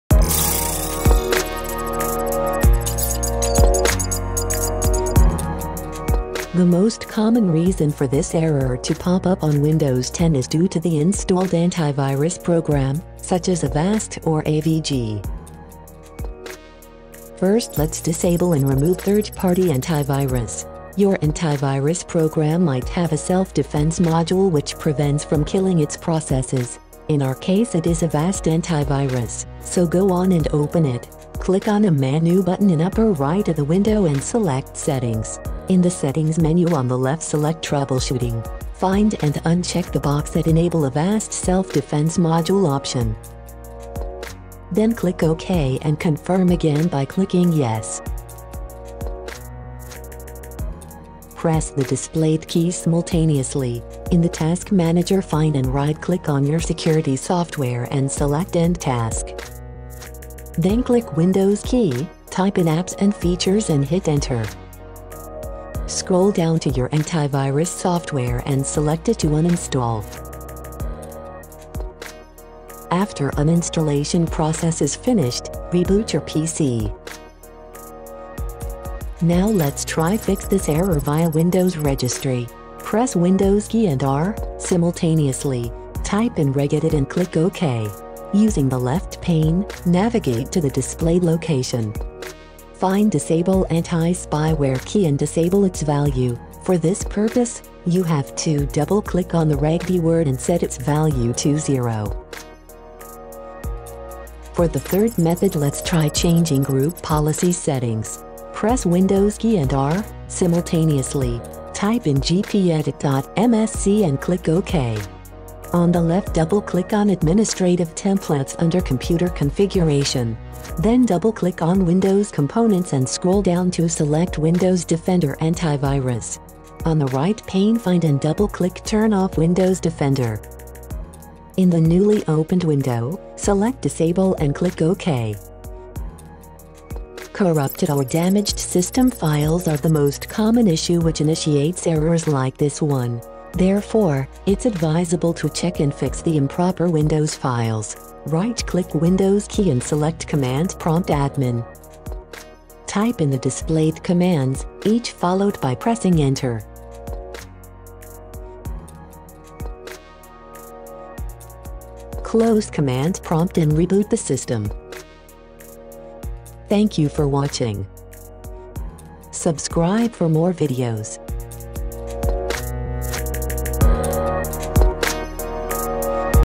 How to Fix Error Code 0x800704ec when Turning On Windows Defender
Error Code 0x800704ec when turning on windows defender is an official Windows error which we have examined thoroughly and compiled a detailed tutorial for you
Windows defender is a built-in Windows operating system security solution which can be affected by other installed 3-rd party anti-virus programs, local group policy settings or corrupted system files. Therefore 0x800704ec error should be resolved immediately.

Method 1 - 00:16
Method 2 - 01:18
Method 3 - 01:43
Method 4 - 02:16

--TEXT TO COPY/PASTE--
Method 2:
For navigating to windows defender location in registry editor:

HKey_Local_Machine\Software\Policies\Microsoft\Windows Defender

Method 4:
Commands for executing in command prompt environment:

sfc /scannow
DISM.exe /Online /Cleanup-image /Scanhealth